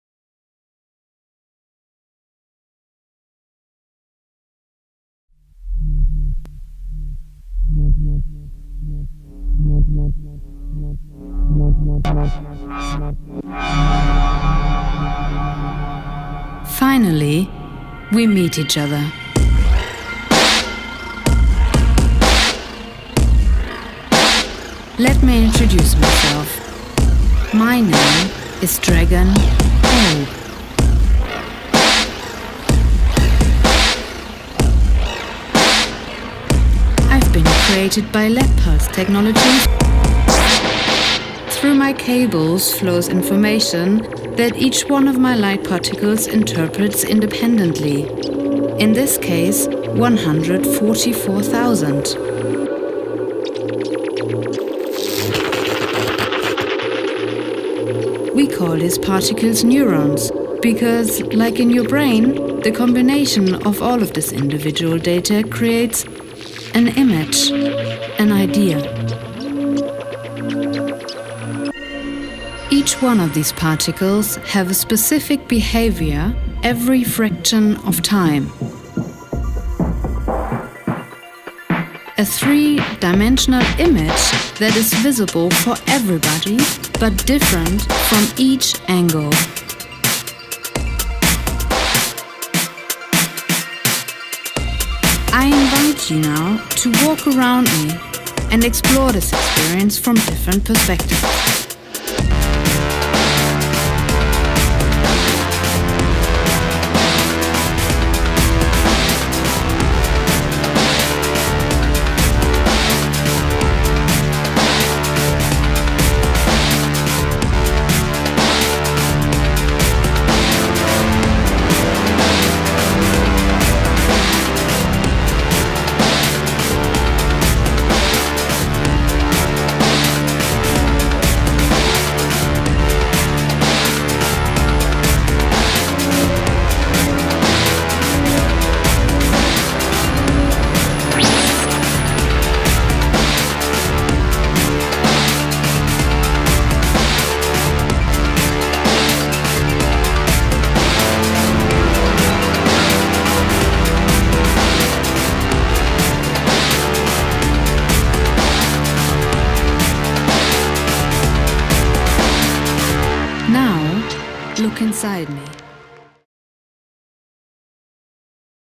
Meet The Dragon
Each one of Dragon's LED particles have a specific behaviour every fraction of time. A three dimensional image that is visible for everybody but different from each angle